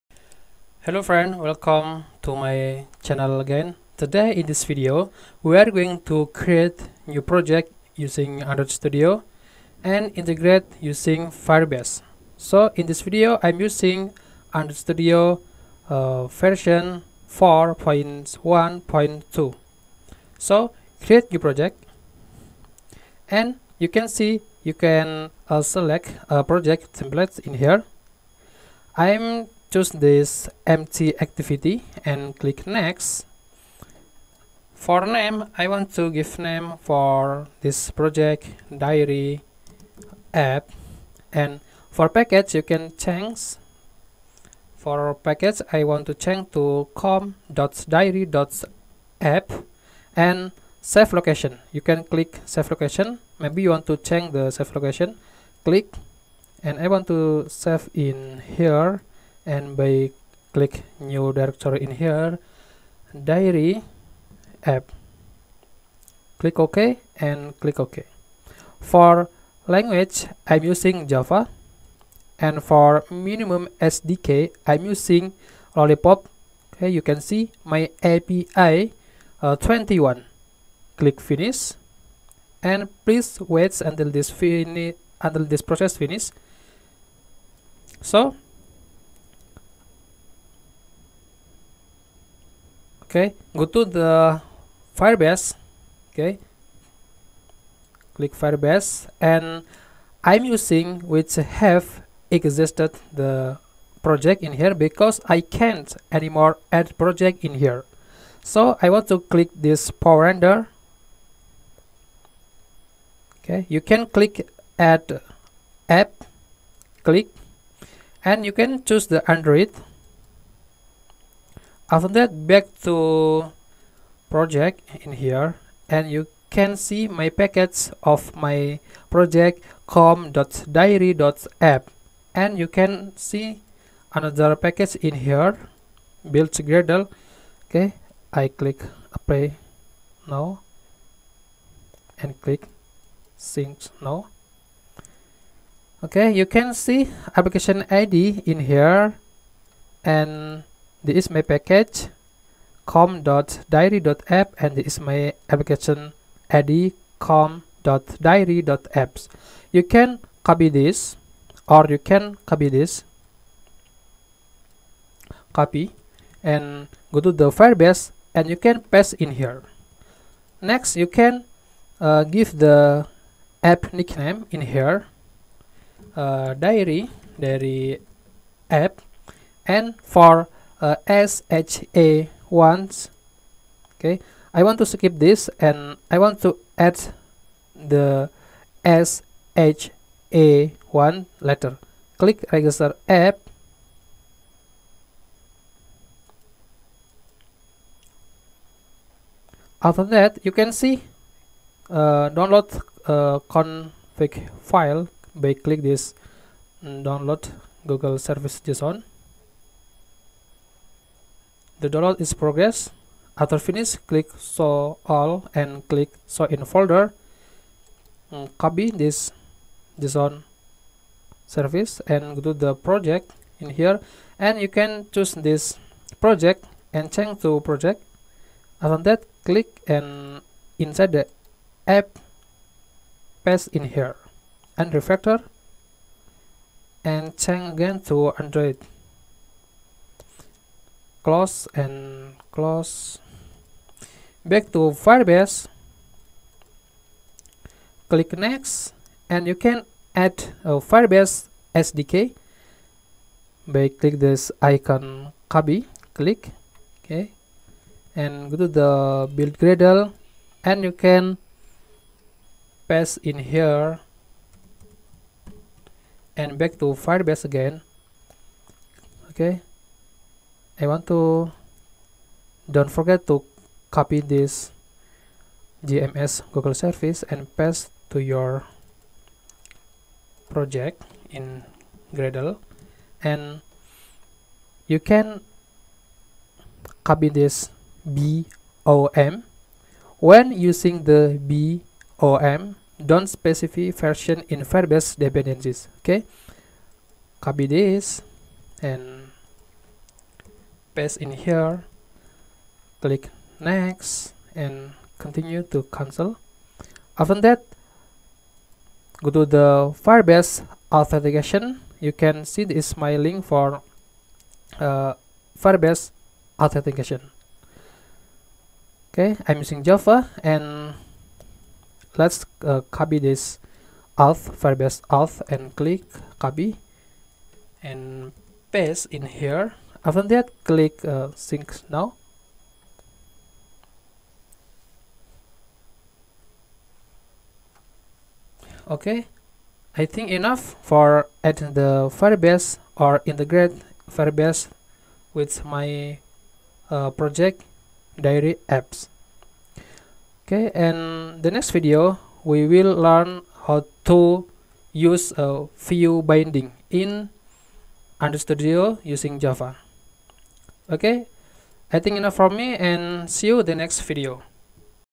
Create New Project and Integrate with Firebase - Diary App part 2
Build diary app using java and firebase in android studio from scratch until finish